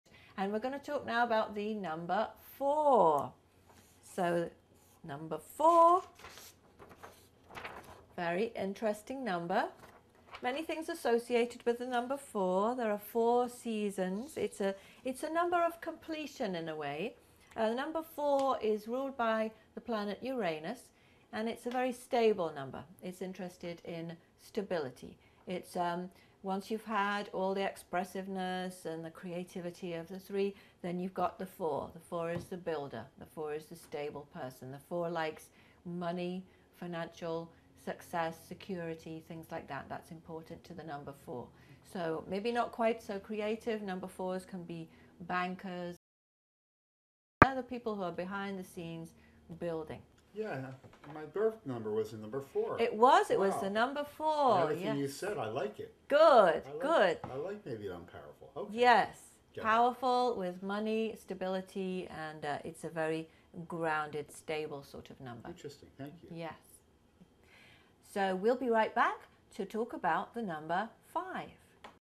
Stability & the Number 4 in Numerology
Stability & the Number 4 in Numerology. Part of the series: Understanding Personal Numerology. The number 4 in numerology is for stability, learn more about the number four and numerology in this free video.